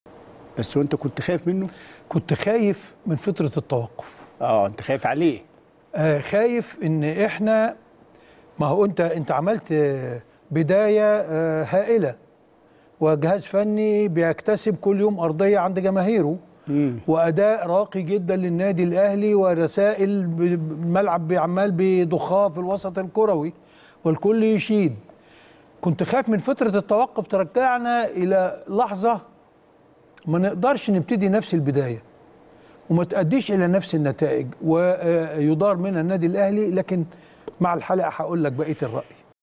القيعي: كان يوجد قلق بسبب توقف الدوري الممتاز لـ يؤثر على مستوي الفريق
التردد: 12341 أفقي.
القمر: نايل سات.
معامل الخطأ: 5/6.
معدل الترميز: 27500